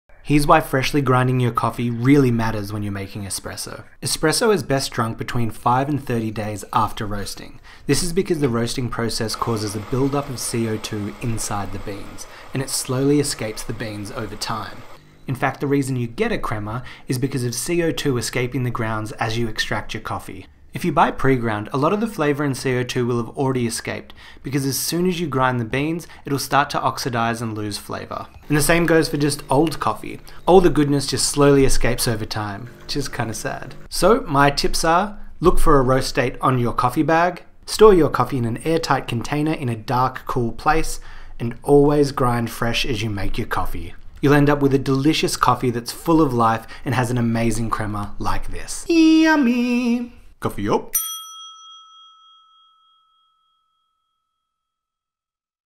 Grinding fresh is key to a great coffee
Grinding you beans just before use is very important to enjoying a good coffee. We are specialists at making great coffee at Folders.co.nz and along with our great partner Sunbeam we explain why.